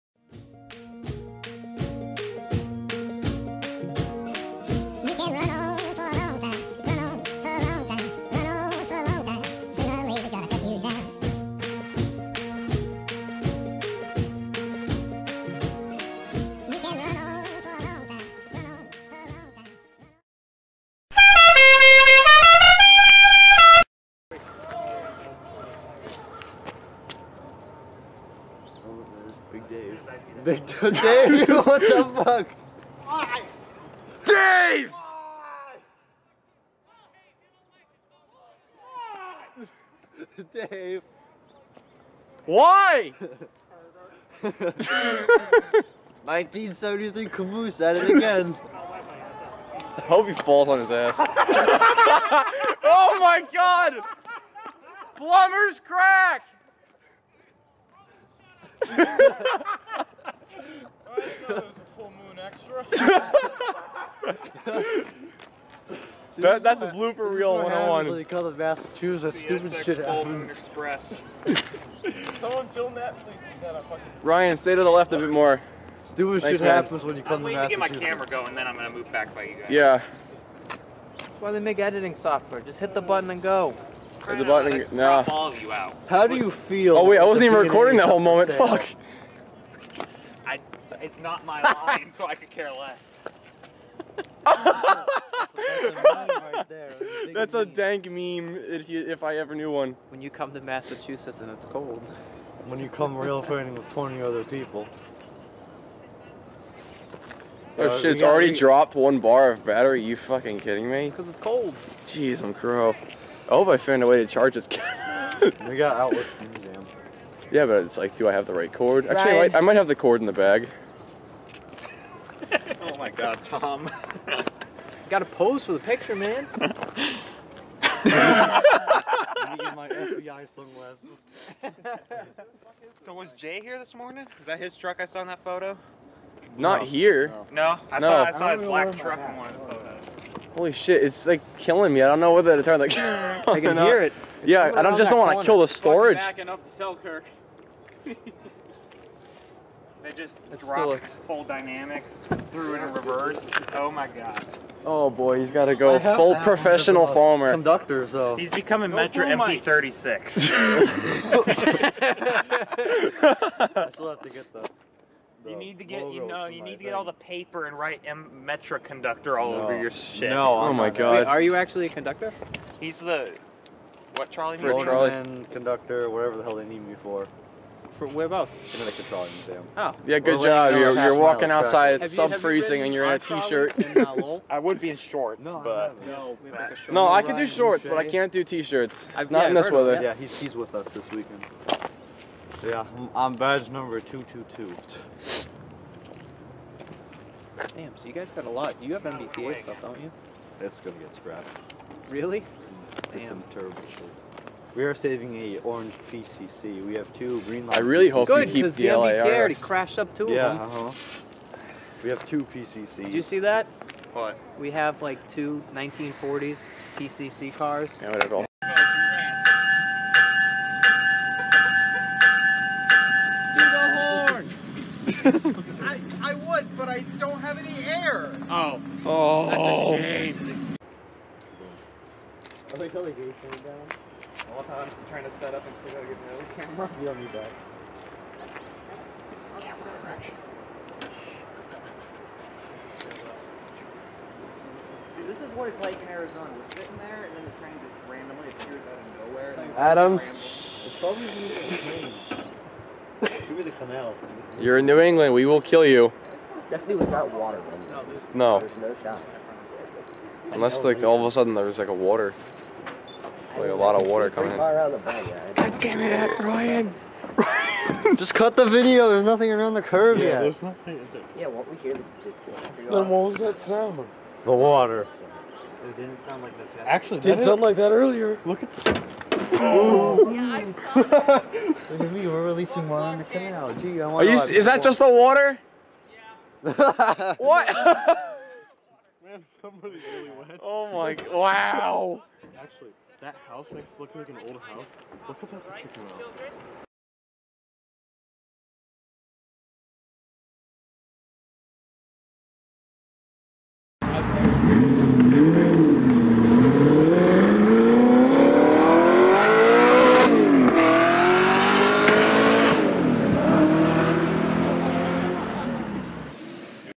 200 Subscriber Special - 2018 Springfield Bloopers
In honor of reaching (and exceeding) the 200 subscriber mark, I decided to stitch together this compilation of non-scripted humor from the best weekend of the year thus far! Be advised, there is a lot of swearing and borderline offensive jokes. This is all just for laughs and not meant to offend anyone.

-LLP